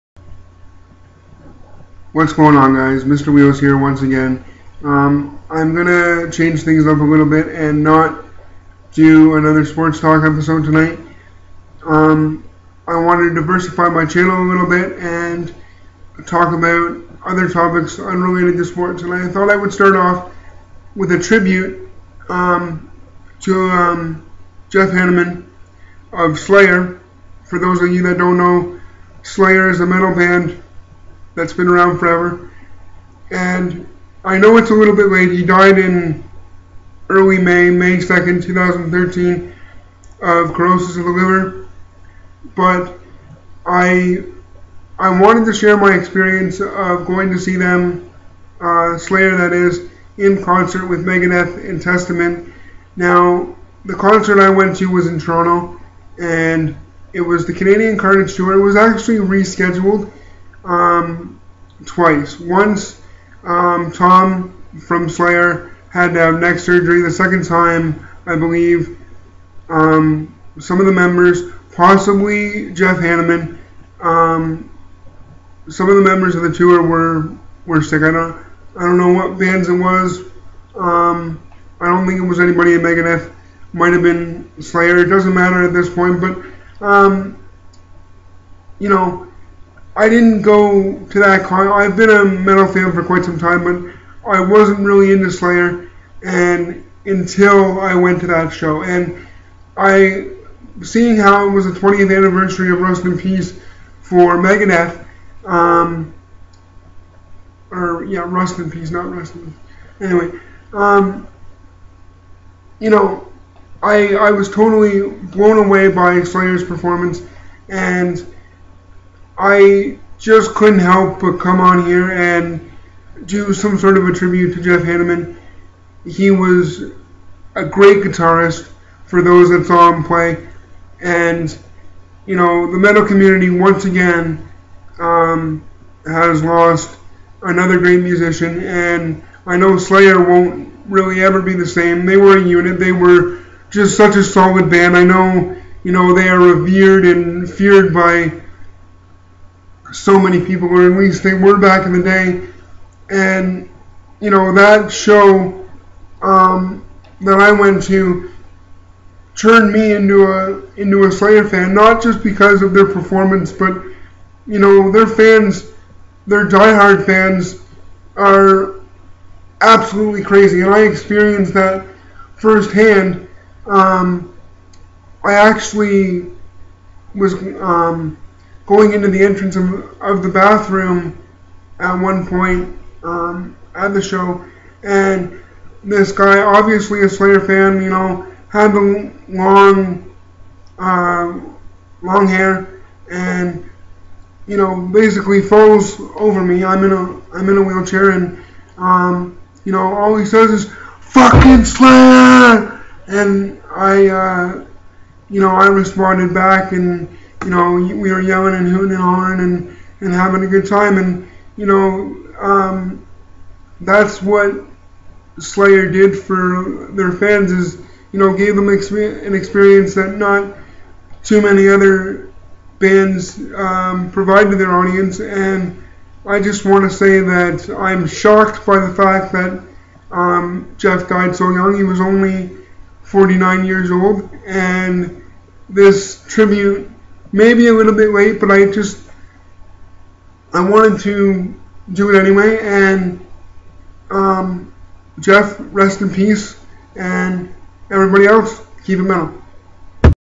Tribute to Slayer Gutiarist Jeff Hanneman
It may have come four months late, but this is a tribute video for Slayer Gutiarist Jeff Hanneman.  We lost a true legend...

R.I.P. Jeff Hanneman \m/